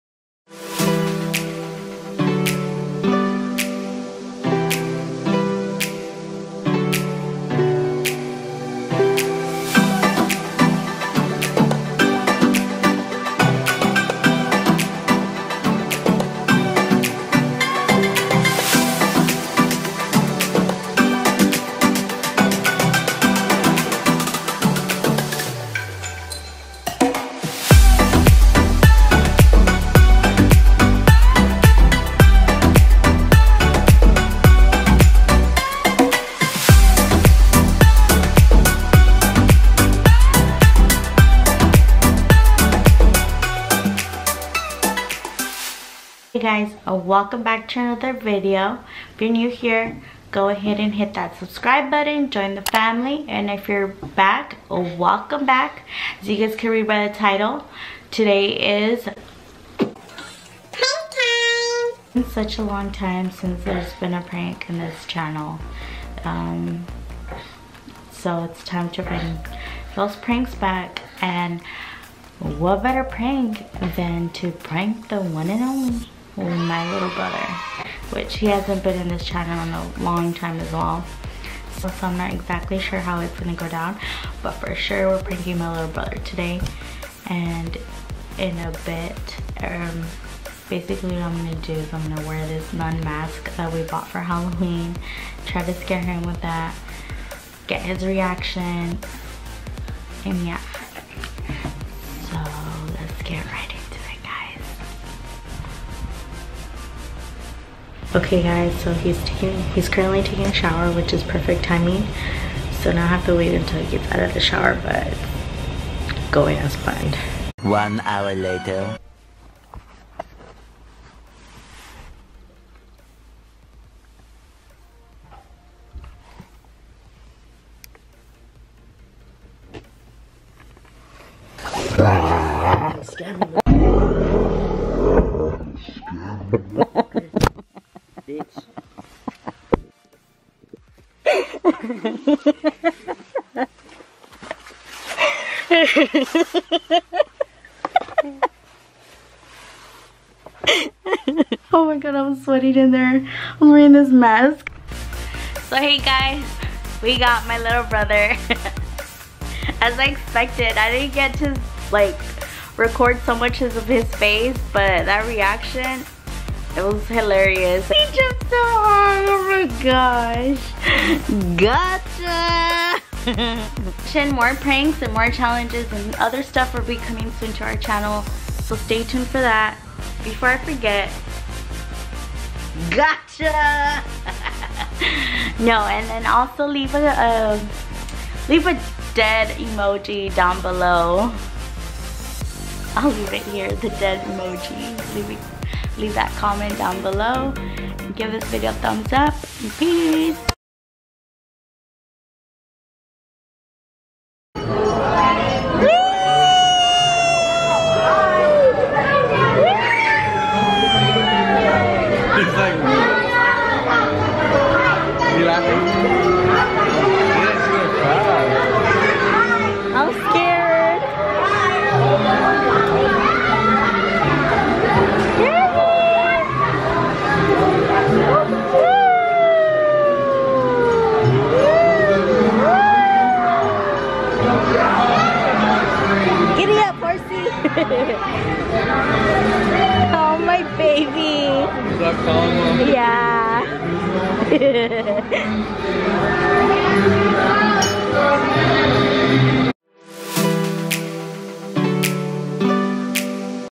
PRANK ON MY LITTLE BROTHER! *HE GOT SCARED*
Hey guys, welcome back! & if your new here WELCOME! Join the Family! Today I prank my little brother ! his reaction was hilarious! enjoy !